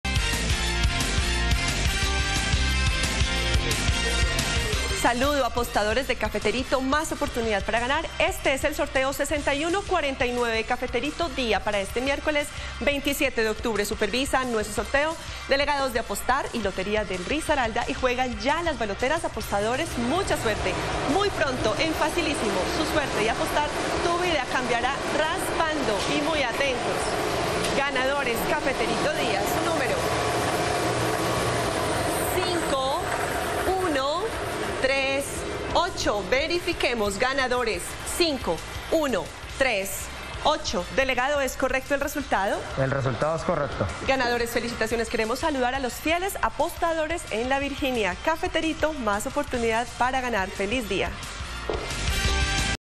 Resultado EL CAFETERITO TARDE del miércoles 27 de octubre de 2021
Hola buenas tardes, te presentamos los resultados de las loterías y chances en Colombia, vamos con el Resultados del Chance Cafeterito Tarde del miércoles 27 de octubre de 2021.

cafeterito tarde
cafeterito dia